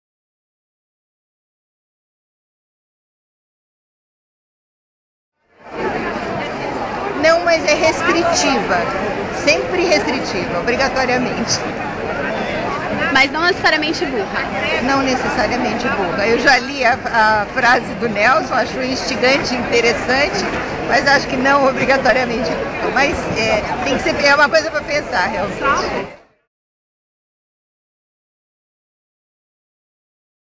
Fanny Moore - Sociologa
Cobertura da FLIP pelo Jornal de Debates - "Toda a unanimidade é burra?"